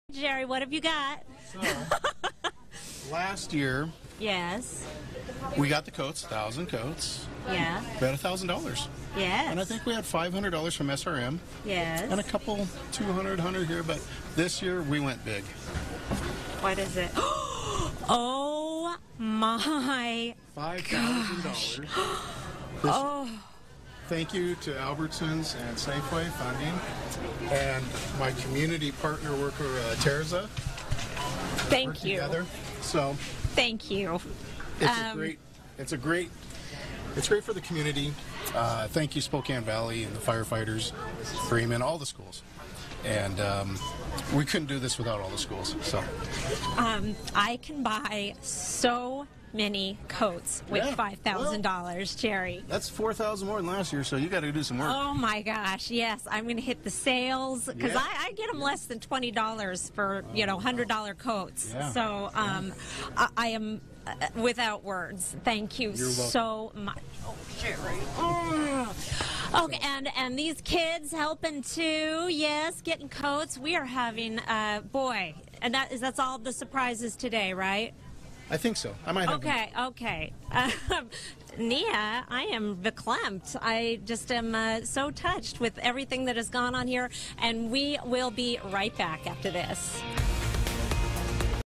Albertsons surprises Coats 4 Kids with $5k donation
The annual Coats 4 Kids donation drive kicked off on Wednesday, but it wasn't just coats being donated.